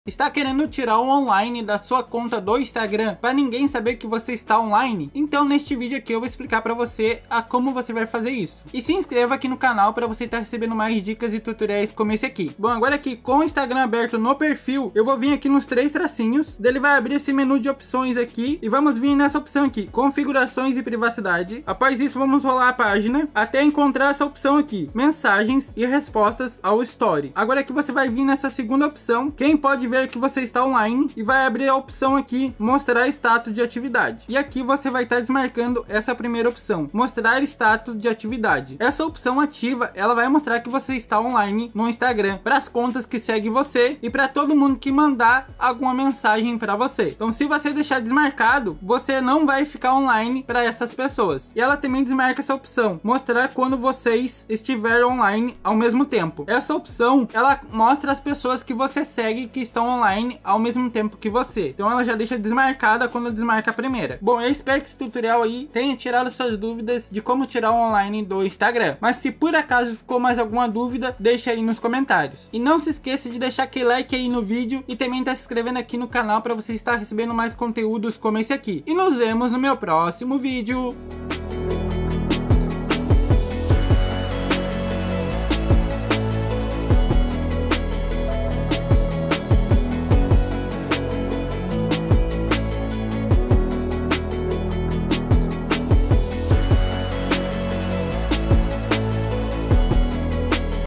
Como TIRAR ONLINE do INSTAGRAM em 2024!
Para você que quer ficar offline e que ninguém saiba que você está no instagram, neste vídeo eu vou explicar o passo a passo de como fazer para tirar o online da sua conta do Instagram. Assim você pode usar sua conta sem que ninguém saiba que você está no Instagram. Confira o tutorial de como tirar o online do Instagram. Caso ainda tenha dúvidas, deixe nos comentários.